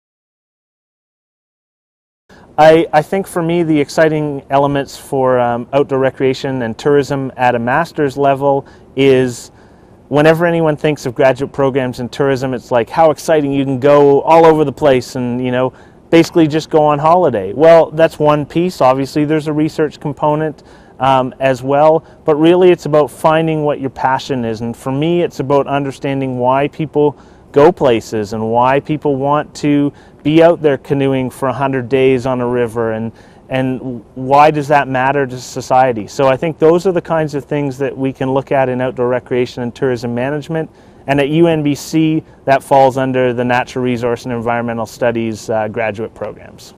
The reason to choose to do graduate work in ORTM in NRES
Dr. Patrick Maher explains why he feels graduate work is rewarding in the Outdoor Recreation and Tourism Management Program of Natural Resources and Environment Studies Graduate Program